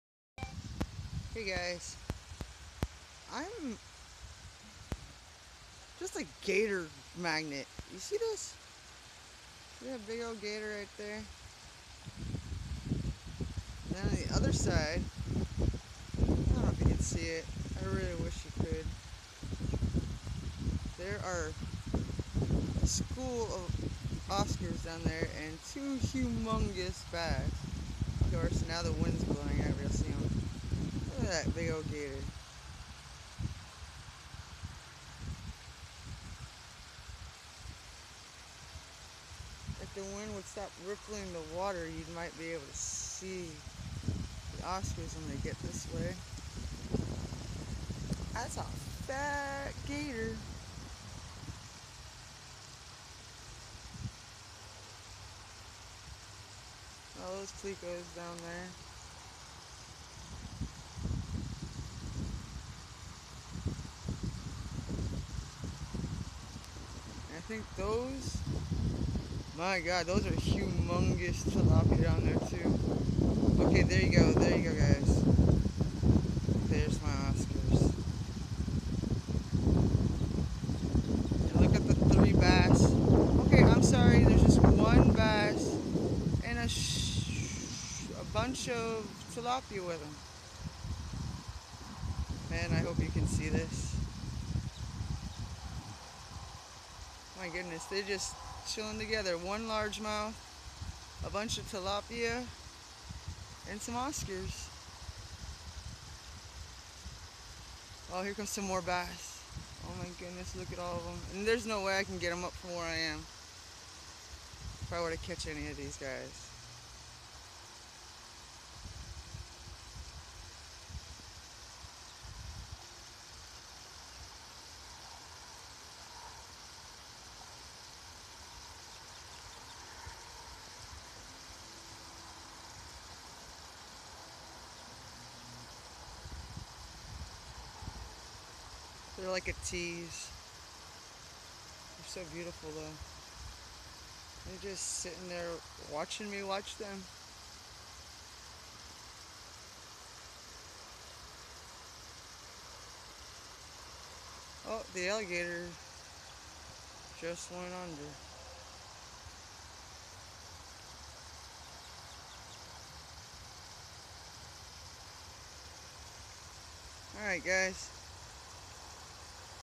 Bass, gator, tilapia and Oscars
Watching fish at a channel lock